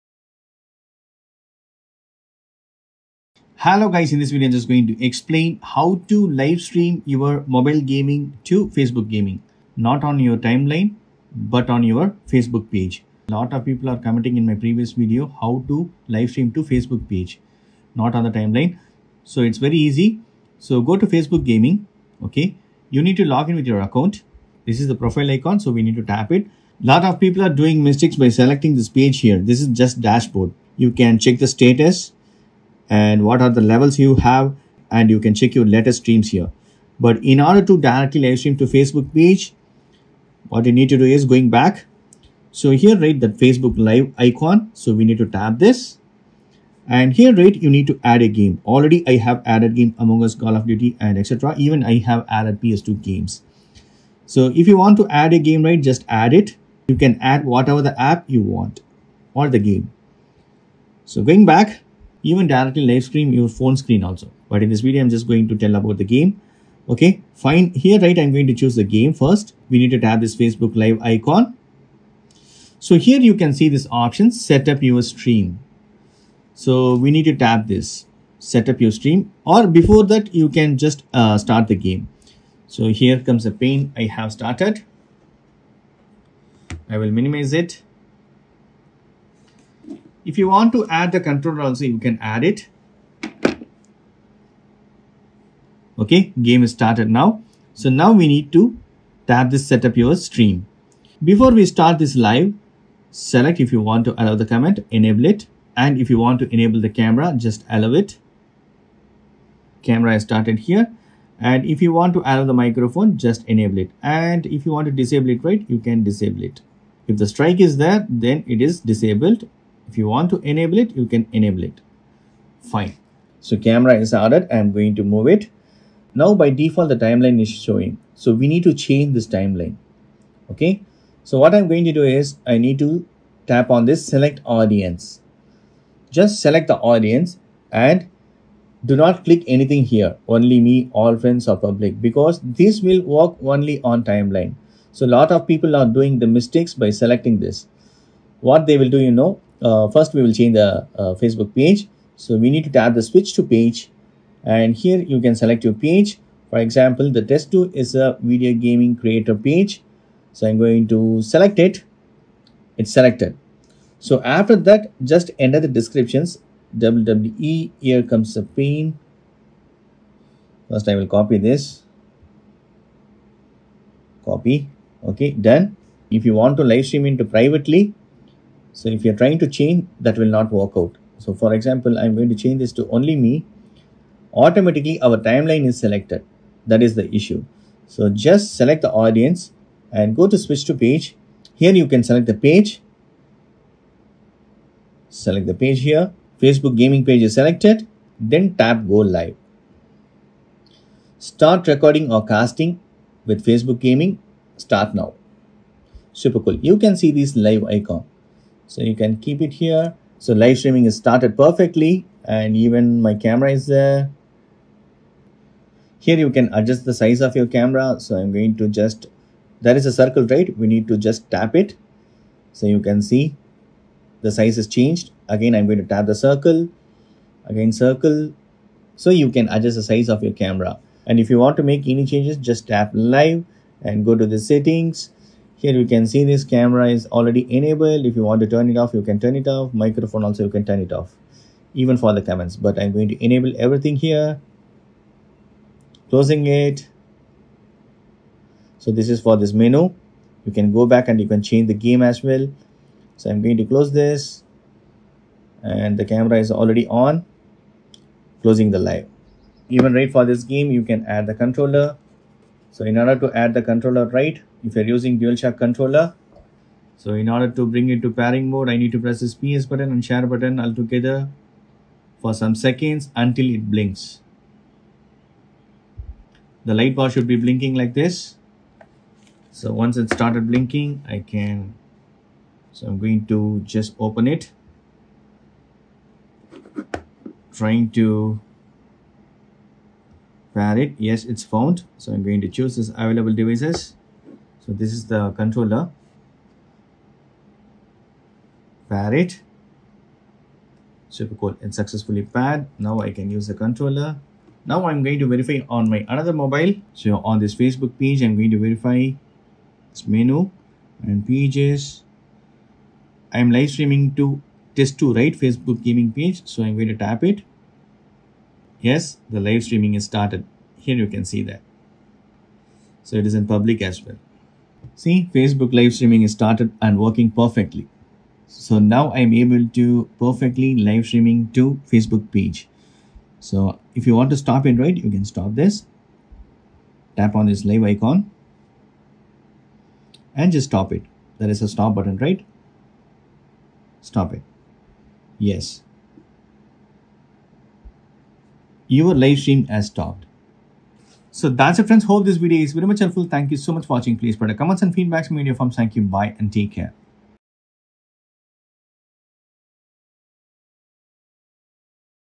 Facebook Gaming : How to Live Stream Mobile Games to your Facebook Page? Not on your TIMELINE
hello guys, in this video, i am explaining that how to live stream your android mobile games to your facebook page and not on your facebook timeline. also i have explained how to add controller to your android mobile.